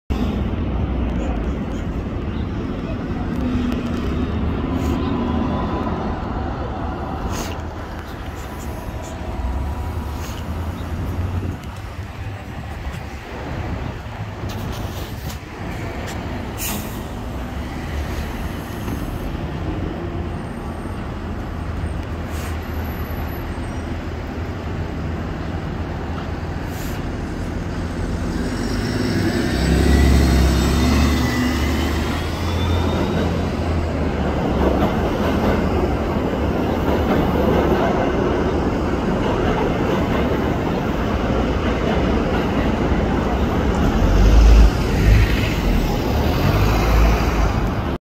(EXCLUSIVE) MTA NYCT Bus: 2022 New Flyer XDE40 #9431 on the M55 at Av Of The Americas & Spring St
Taken on February 11, 2022.

Another new xcelsior..